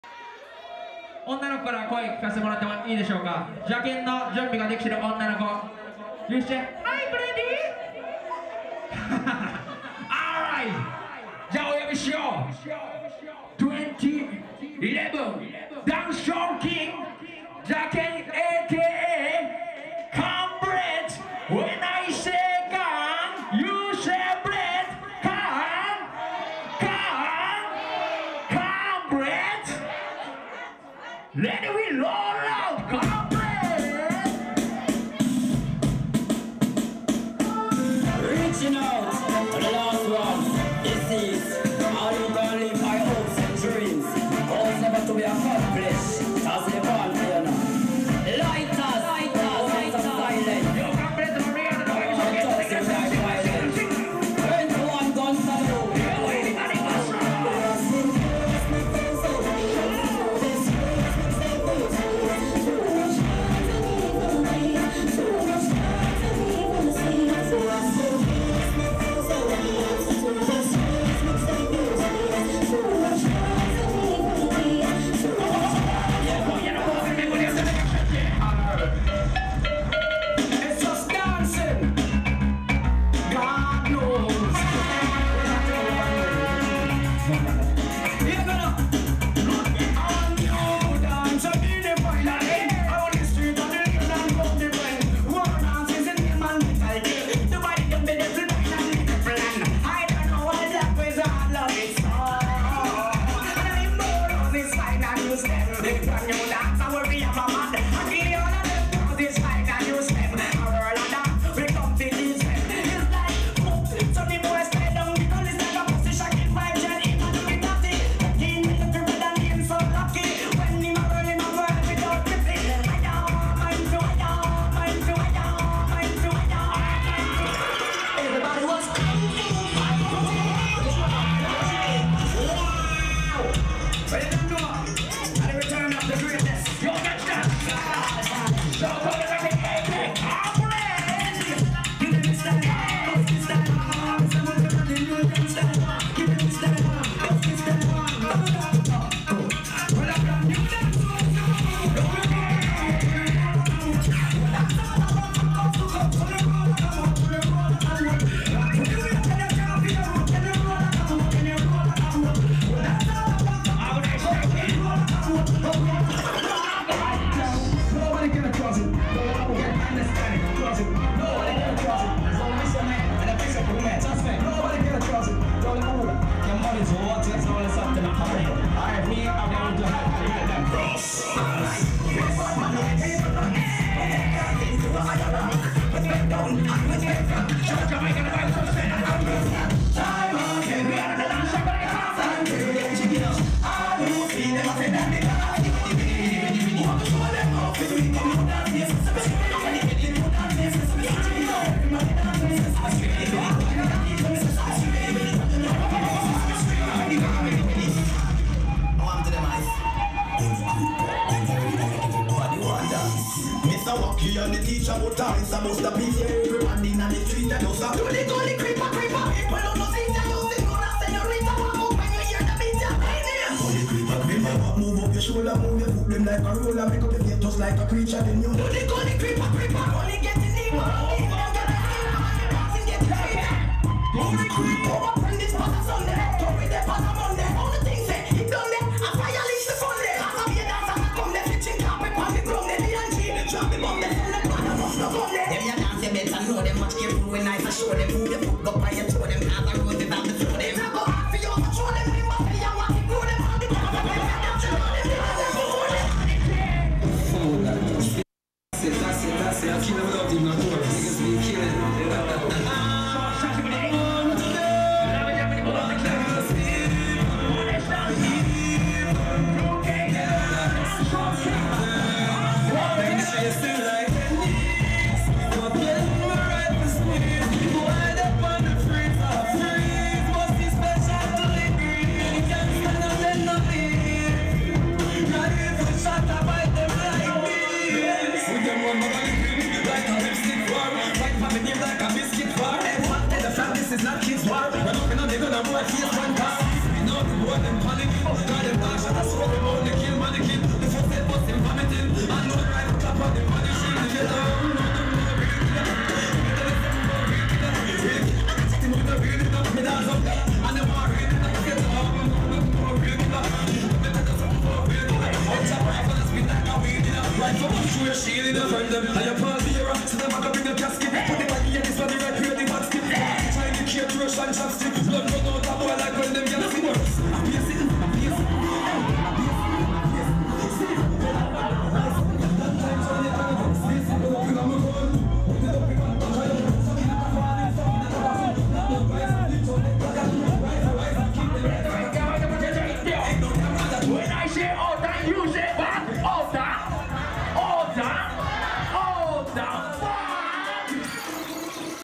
CORN BREAD【SHOWCASE】RAM JAM DANCEHALL "Bogle Memorial 2012"
2012/1/19(木)RAM JAM DANCEHALL"Bogle Memorial"@池袋bed
 [GUEST DANCER]CORN BREAD /CHERRY BLOSSOM /HIGHER GIRLS /HELTY BADDY GIRLS /SWAY n MOEKO/MILKY/TOMO/SESS
[GUEST SOUND] BABY-NON rep.BADDY 45 /MAD DAWG [HOST SOUND] DJ HATA-K /NITE WARRIOR